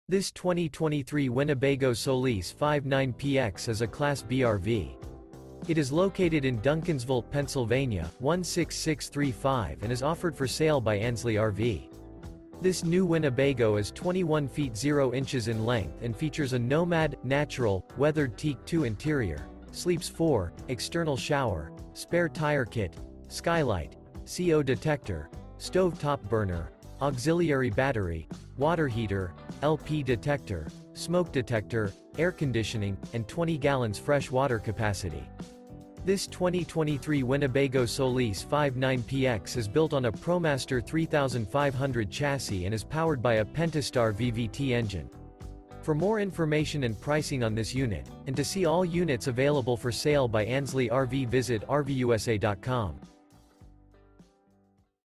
Breathtaking 2023 Winnebago Solis 59PX Class B RV For Sale in Duncansville, PA | RVUSA.com
This 2023 Winnebago Solis 59PX is a Class B RV. It is located in Duncansville, PA 16635 and is offered for sale by Ansley RV. This New Winnebago Is 21' 0" in length and features a Nomad/Natural/Weathered Teak II interior, sleeps 4, CO Detector, Skylight, Spare Tire Kit, LP Detector, External Shower, Stove Top Burner, Smoke Detector, Refrigerator, Air Conditioning, Auxiliary Battery, and 20 gal fresh water capacity. This 2023 Winnebago Solis 59PX is built on a ProMaster 3500 chassis and is powered by a Pentastar VVT engine.